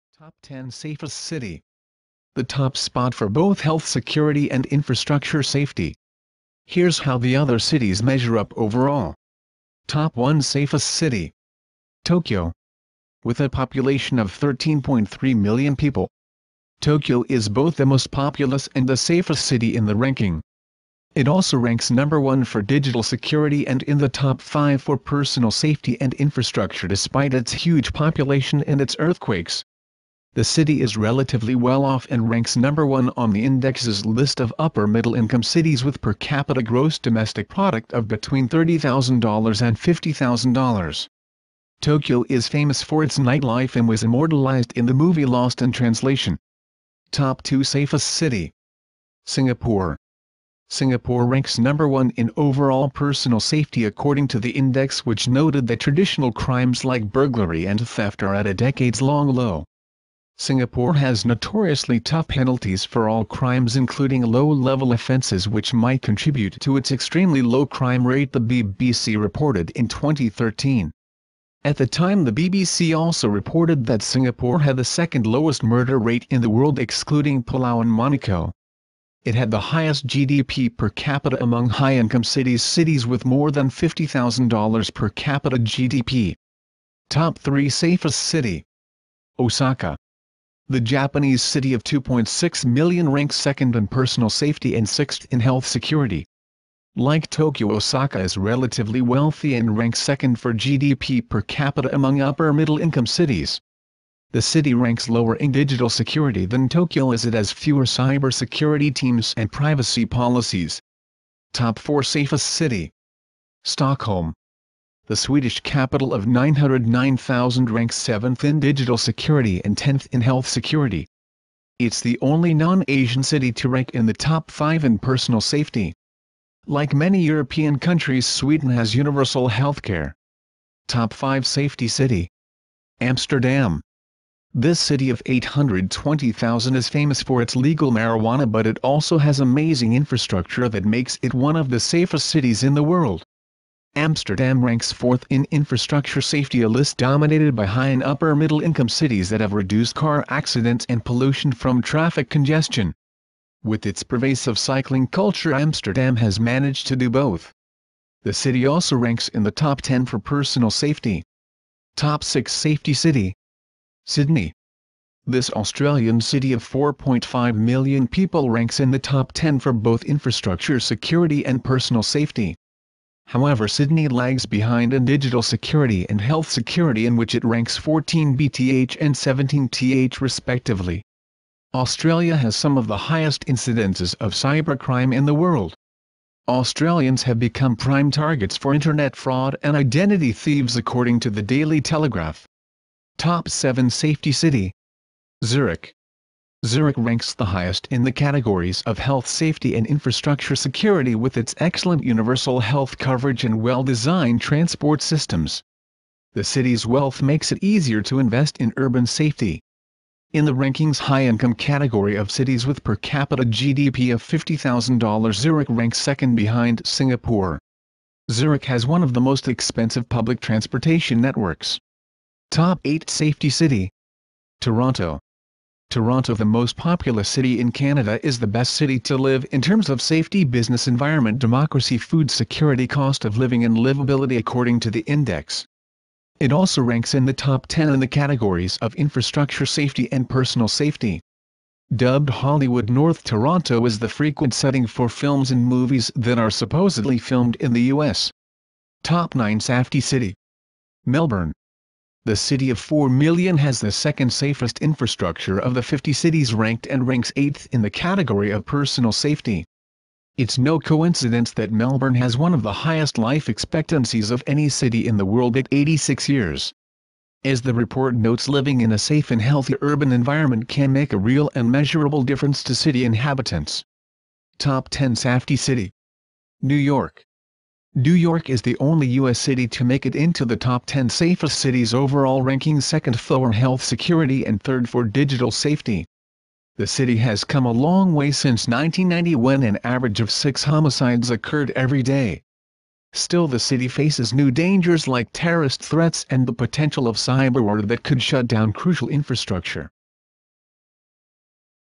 10 safest cities Documentary
A city's friendliness may bring in tourists, but it's a city's safety that plays a role in who wants to come back. So for travelers for whom safety is a top priority, TripAdvisor's 2014 Cities Survey may be a list of interest before booking that next flight.
The study, compiled by Brainbox Research, looked at 75,000 surveys, each ranking the 40 cities that brought in the largest number of international travelers. Cities were judged on factors like friendliness, value and ease when it comes to getting around, among other criteria